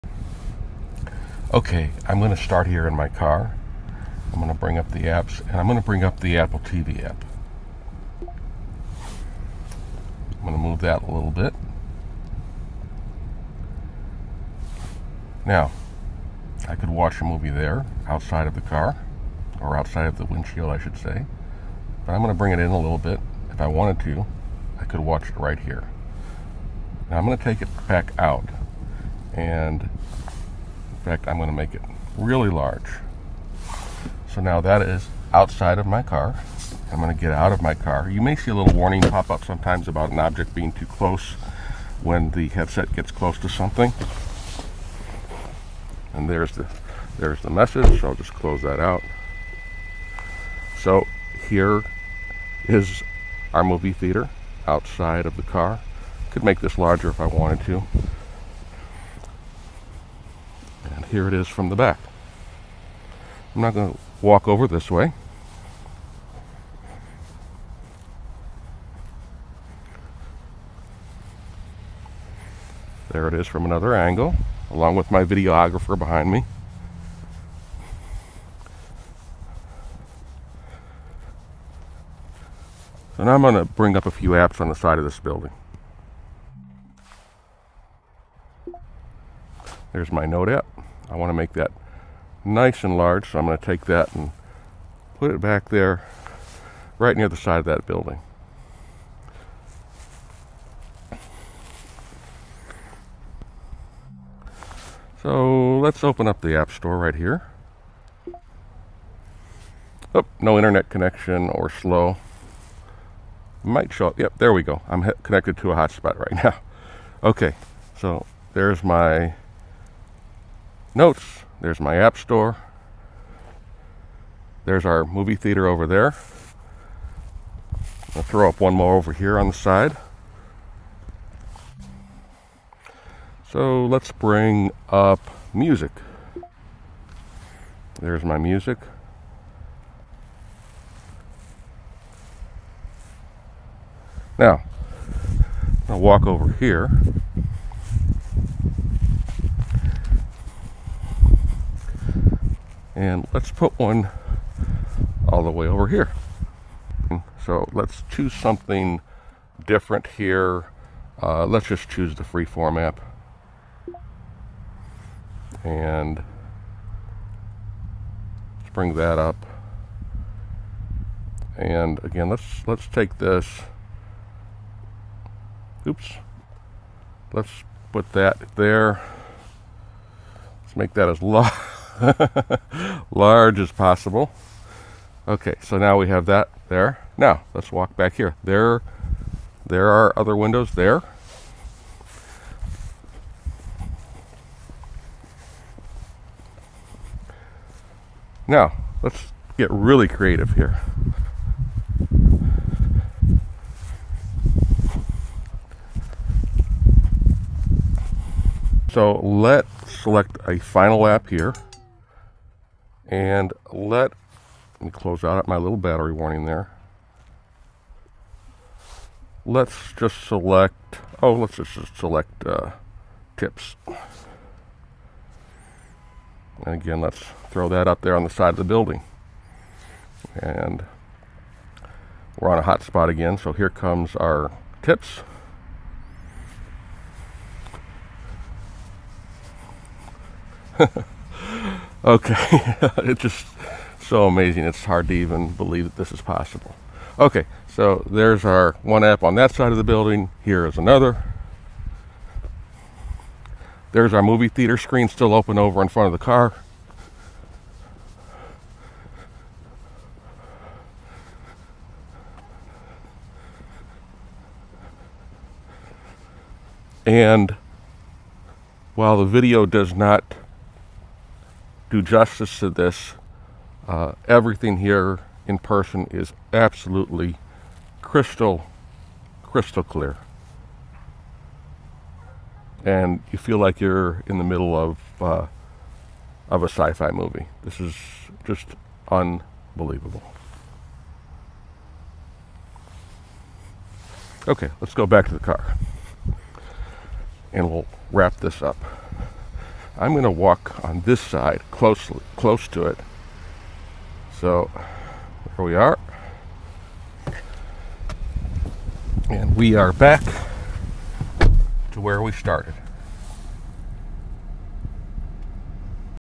Apple Vision Pro - This Demo will Blow Your Mind!
The Apple Vision Pro is like tech out of a sci-fi movie. A couple of things to note.
- I made this video to show some of the tech this device is capable of. While what I'm showing here has no practical application, the technology most certainly does, and I'll be covering that in future videos. The fact that you don't need to create boundaries for the Vision Pro like you do with other AR/VR headsets, opens up new worlds of possibilities for spacial computing and AR/VR.
- I've not yet tried to use this in the passenger seat with the car moving. I may cover that in a follow-up video.

I'm going to be publishing a ton of completely unique content about the AVP in the coming weeks.